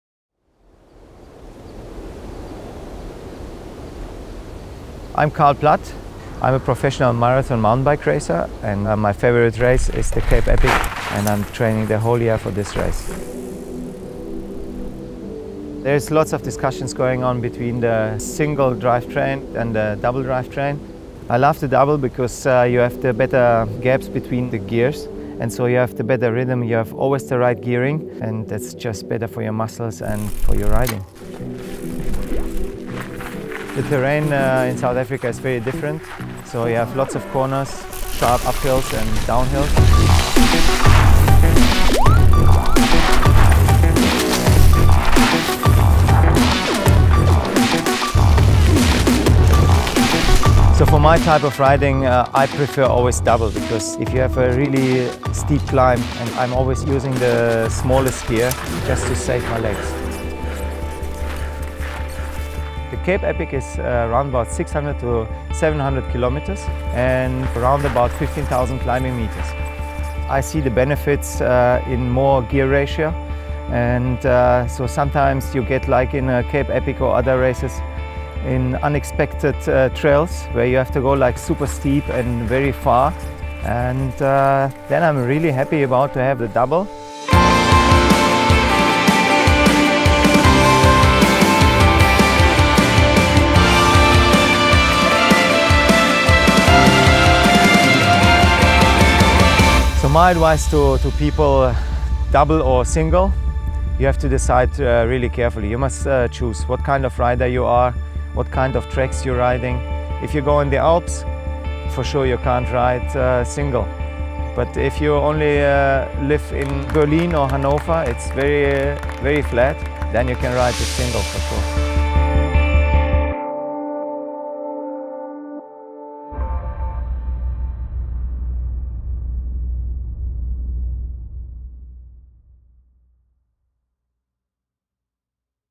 Single or double drivetrain? See why Karl Platt rides 2x11 | SHIMANO
There is no single truth. That’s why Shimano created this interactive and easy-to-use Drivetrain Advisor.